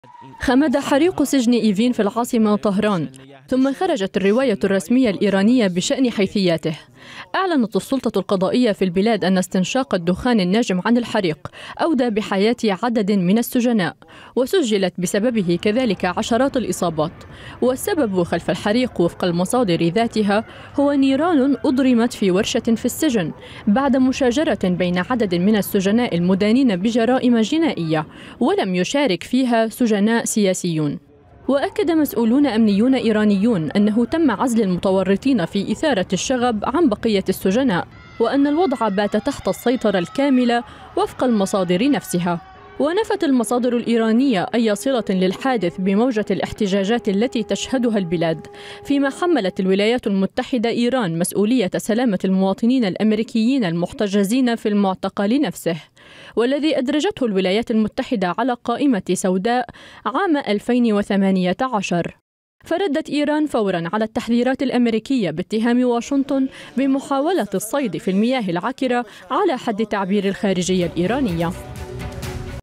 وسائل إعلام إيرانية: 4 قتلى و61 جريحا في الحريق الذي شهده سجن إيفين في طهران
أفادت وسائلُ إعلام إيرانية رسمية، بمقتل أربعة سجناء وإصابة أكثرَ من ستين آخرين في الحريق الذي اندلع في سجن إيفين في العاصمة طهران.
وكانت مصادرُ أمنية إيرانية قد أفادت بعودة الهدوء إلى السجن بعد الحريق والاضطرابات التي تسببت فيها عناصرُ إجرامية، على حد تعبير المصدر